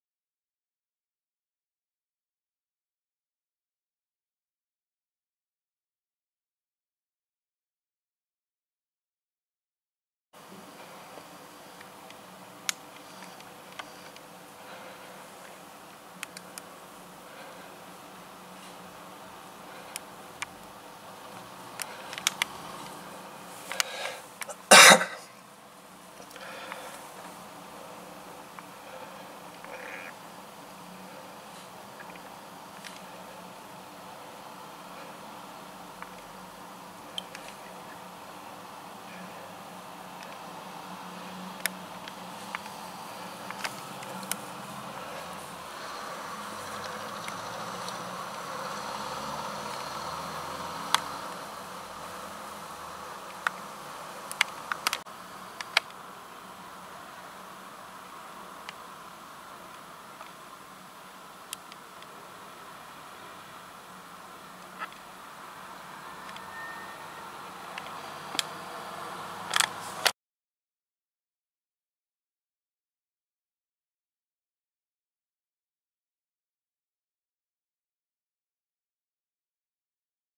TWO CLASSIC VW BEETLES
THE FIRST BEETLE SEEMS TO BE A USA IMPORT LHD WITH SOME COOL FEATURES AND THE 2ND ONE IS PEPPERMINT DREAM CUSTOM BEETLE THAT SOMEONE WON AT A LOCAL VW SHOW!